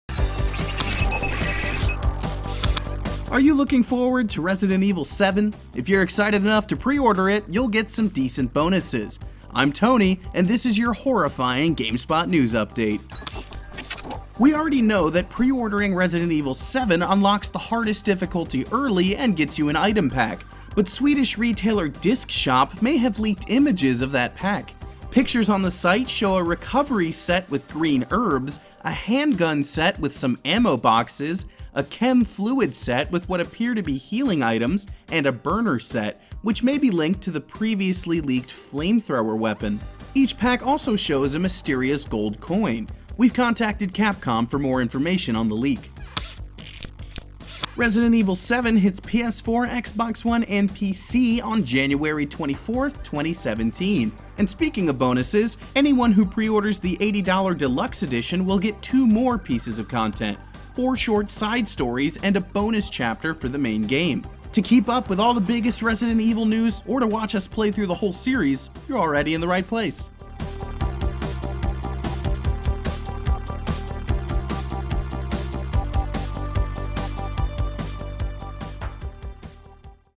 Resident Evil 7 Preorder Bonus Images Leaked - GS News Update
Resident Evil 7 was announced alongside its preorder bonus DLC that unlocks the hardest difficulty early and grants players with an item pack. Now, images of the pack may have been leaked through Swedish retailer Discshop.